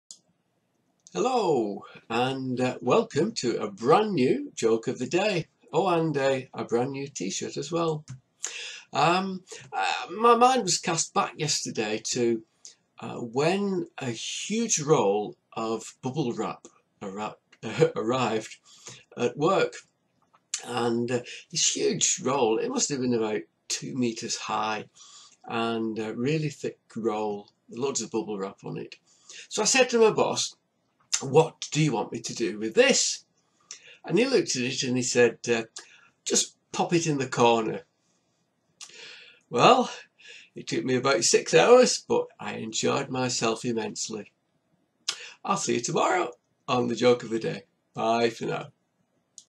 The Bubble Wrap
What happened when a huge roll of bubble wrap arrived at the office